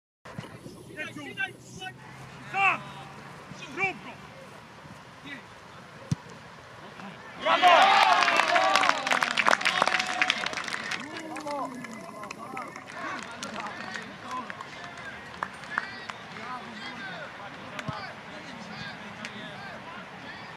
Sęp - ULKS Gołąbek, 2020.08.30: bardzo ładne uderzenie Damiana Grzyba i gol na 3:0
W 42' minucie spotkania Damian Grzyb popisał się bardzo ładnym uderzeniem z 18. metra, zdobywając gola dającego Sępowi Żelechów prowadzenie 3:0.